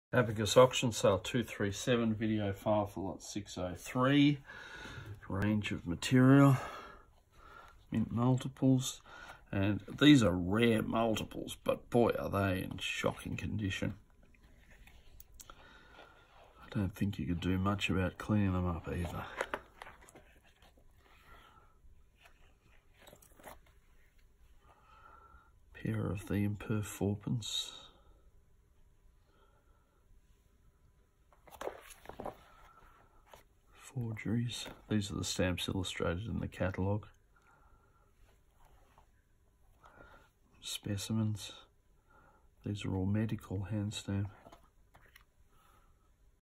Auction 237 Lot 603
Handy range with Imperf 1/- brown used, 1860 4d blue pair unused, 4d carmine & 6d violet both unused, 1885-93 mint set & 1888 New Colours mint, W[crown]A mint set, V/Crown mint to 10/- plus £1 orange CTO with Perth cds & another with concentric-circles, Crown/A mint to 5/-, 'Medical' Handstamp 1d x5 & 2d x2, various 'SPECIMEN' Handstamps to 1/- x2, Coolgardie Cycle Camels pairs (horrid but rare multiples) & Bicycle duo, Telegraph 1d & 6d (a few trimmed perfs) mint, etc, condition generally fine to very fine. A good lot. (Qty 94)